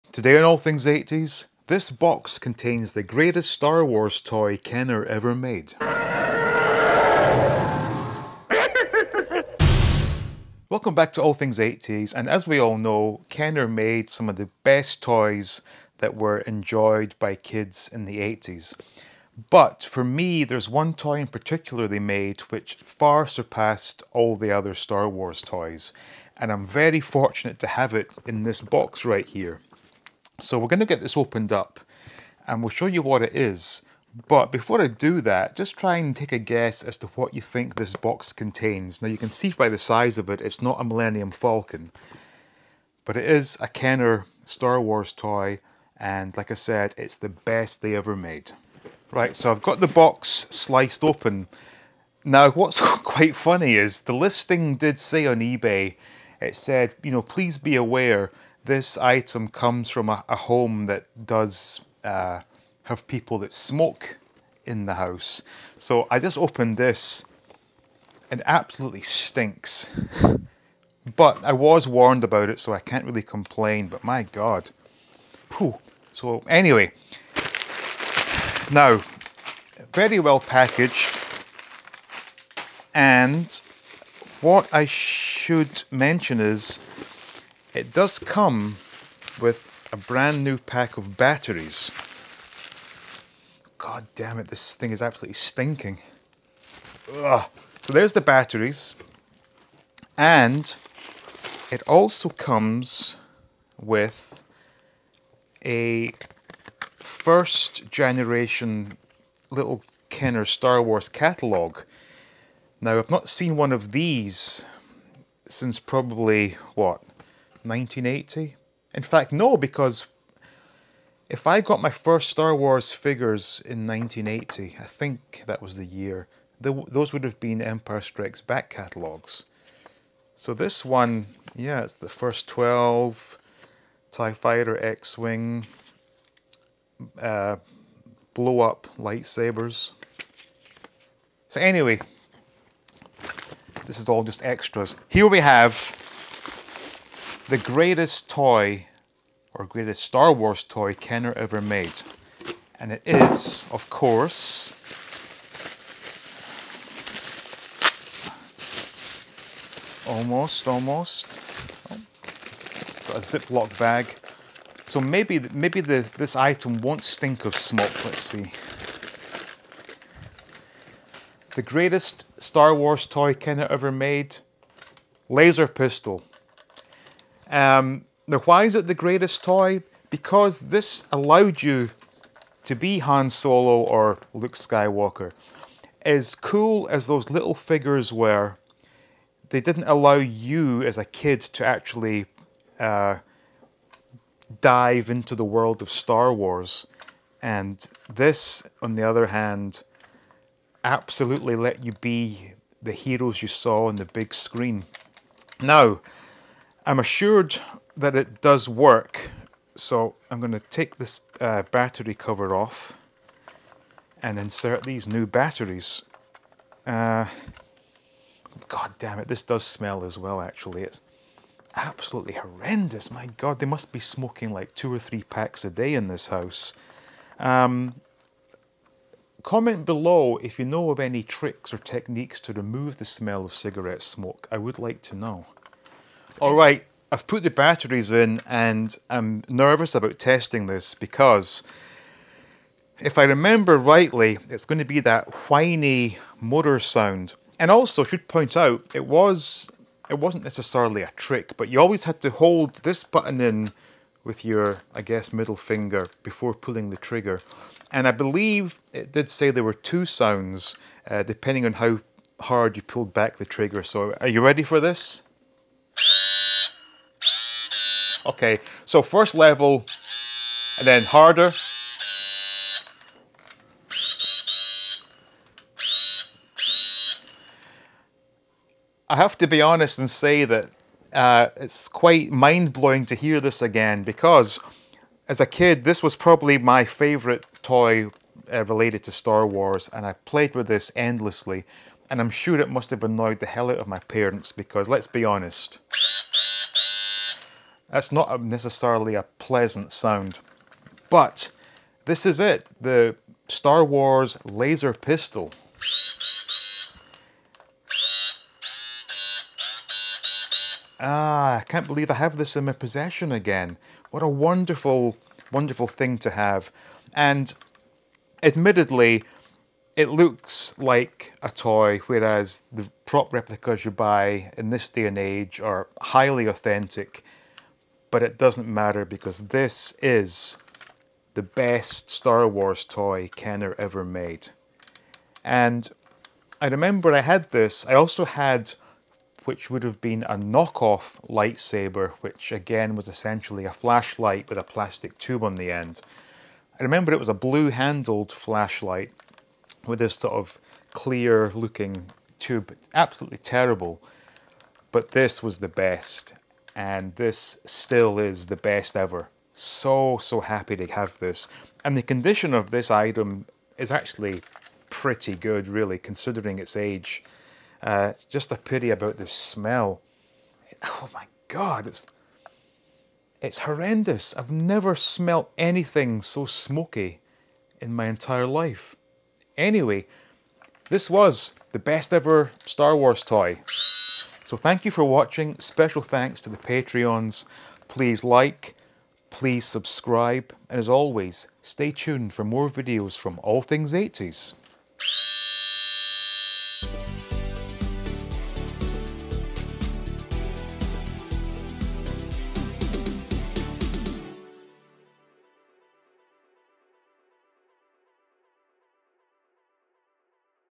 IS THIS THE GREATEST STAR WARS TOY EVER?
I just got hold of what can only be described as the greatest Star Wars toy Kenner ever made.